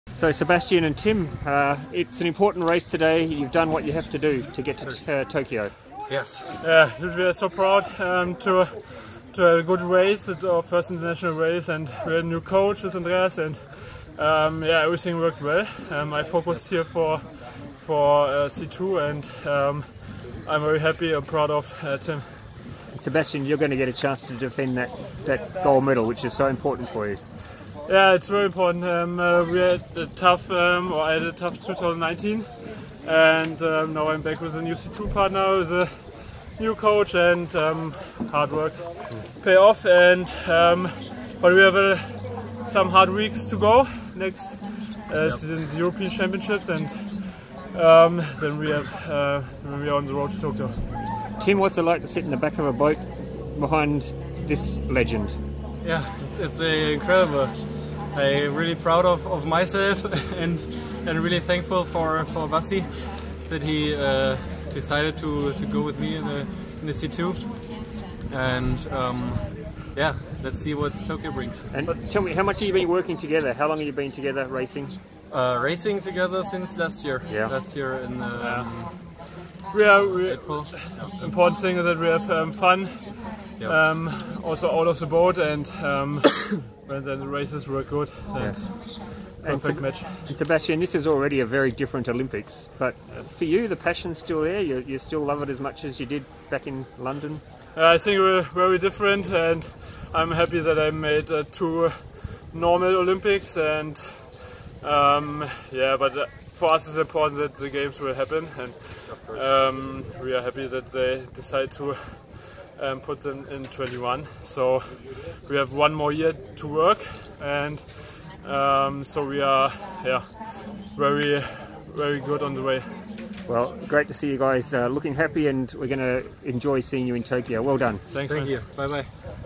Sebastian BRENDEL & Tim HECKER / C2 1000m Gold - 2021 ICF Canoe Sprint World Cup 1 Szeged
Germany's Sebastian Brendel will have a chance to defend his C2 1000 @Olympics gold medal in @Tokyo2020 after winning the world cup final with Tim Hecker in Szeged